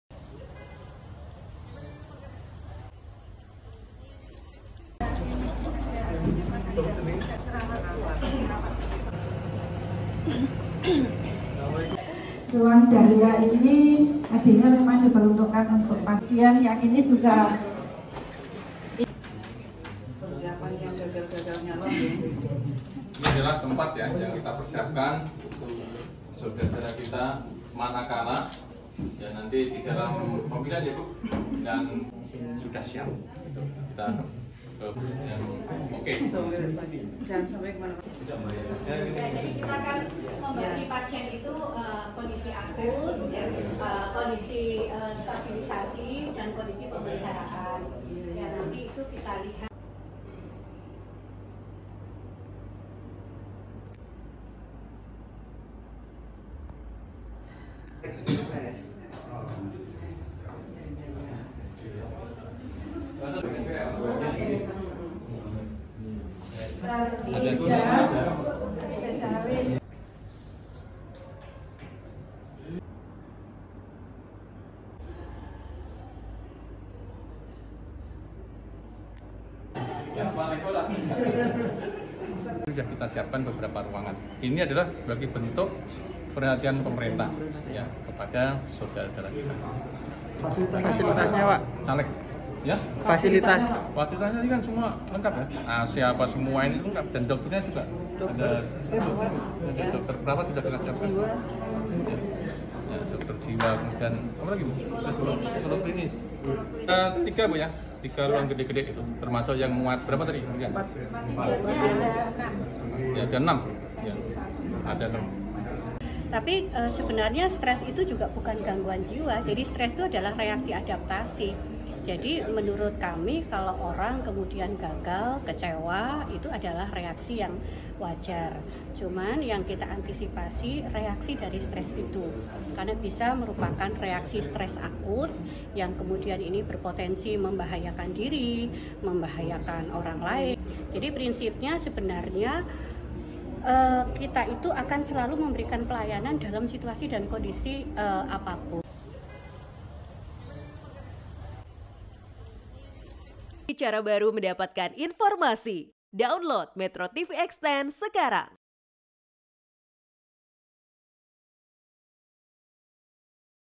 RSUD Wonosari Siapkan Ruang Rehabilitasi Untuk Caleg Gagal
METRO TV JATENG & DIY, Guna Mengantisipasi Adanya Calon Anggota Legislatif Yang Depresi Akibat Gagal Menjadi Wakil Rakyat Rumah Sakit Umum Daerah Atau Rsud Wonosari Kabupaten Gunungkidul Yogyakarta Telah Menyiapkan Ruangan Khusus Untuk Perawatan Termasuk Tenaga Kesehatannya